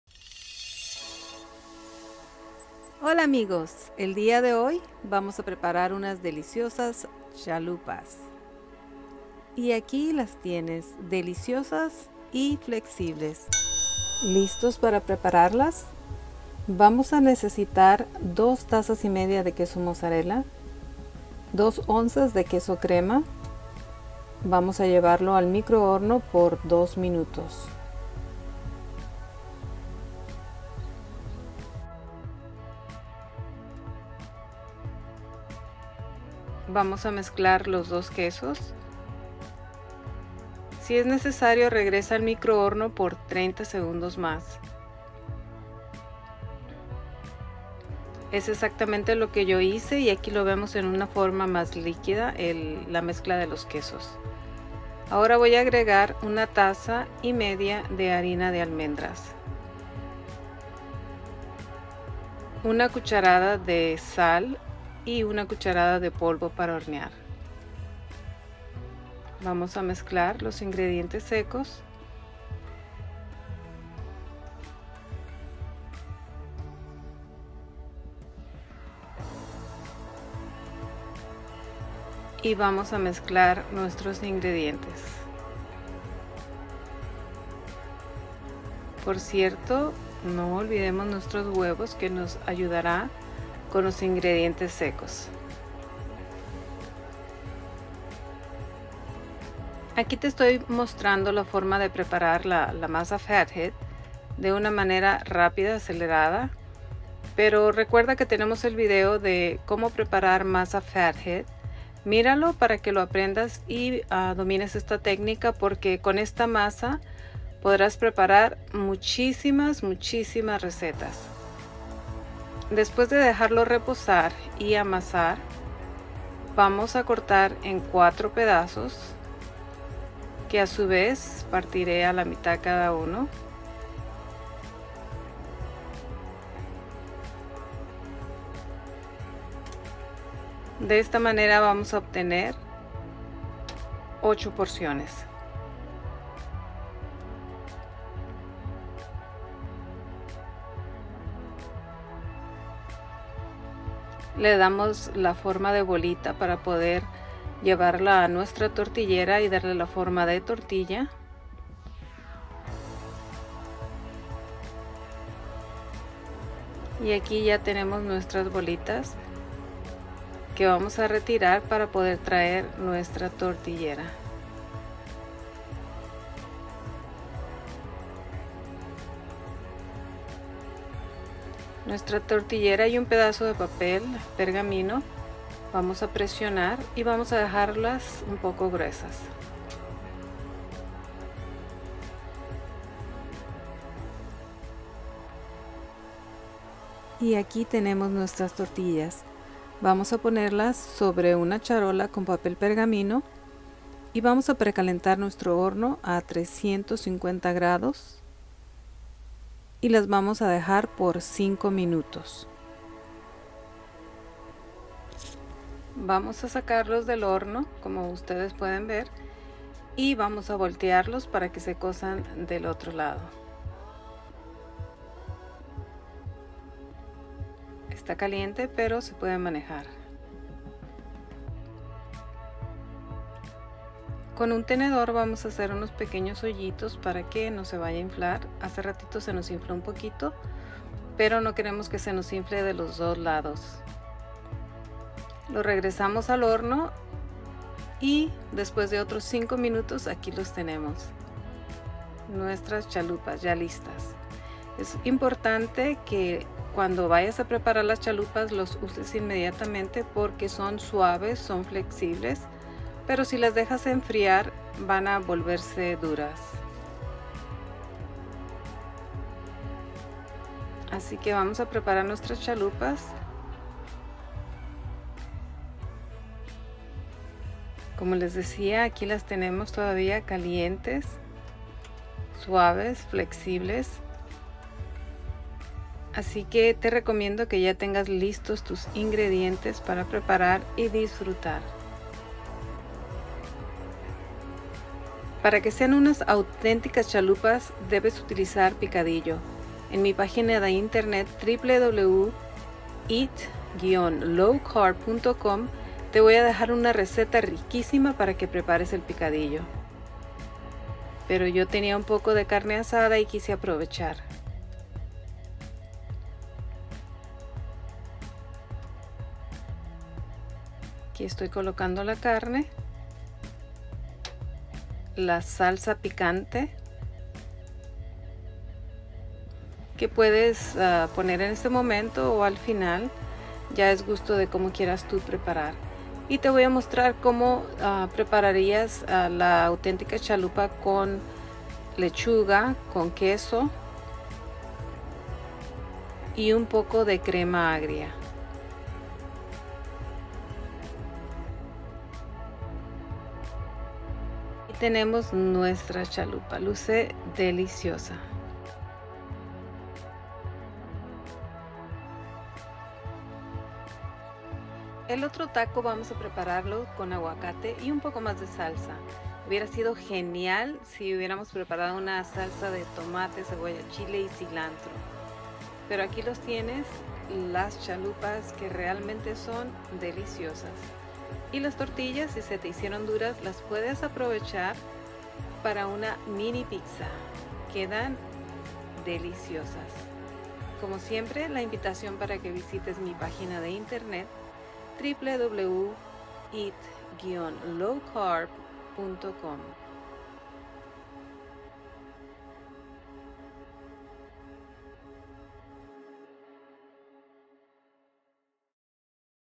Chalupas Keto
Disfruta de unas deliciosas chalupas a base de la masa Fathead, también puedes usar para prepararte una mini pizza o tortillas haciéndolas mas delgadas